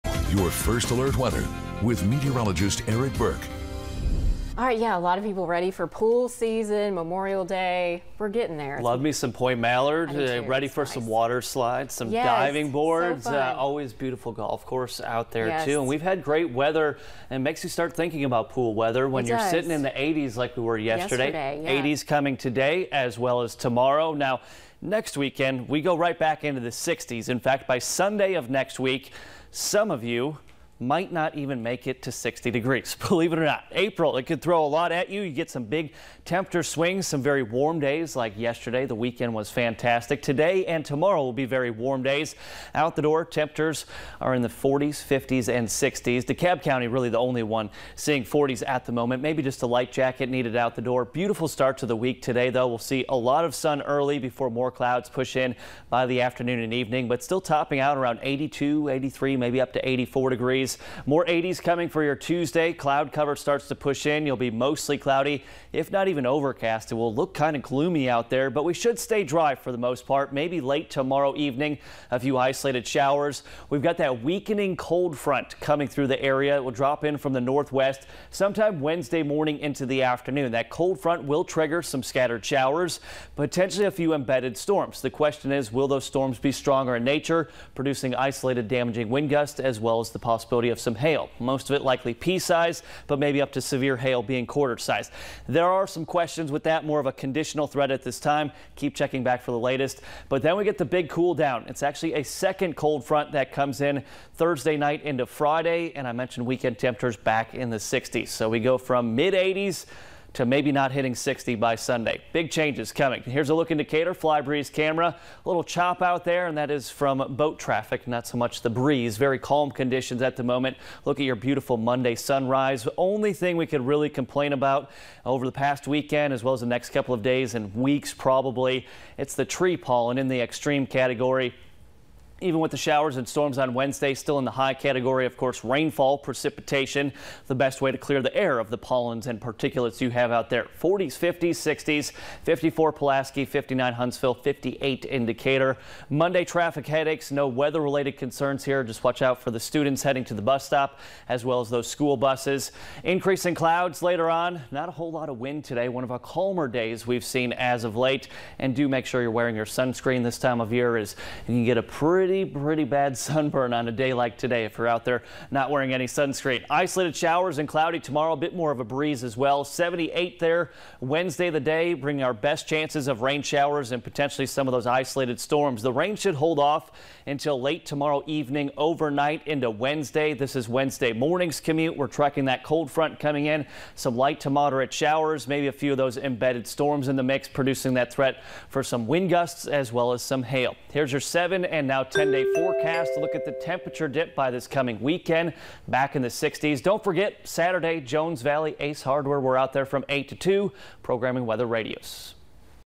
48 First Alert Forecast: Warm 80s to start the week with rain and storms expected Wednesday
First Alert Meteorologist Eric Burke with your Monday forecast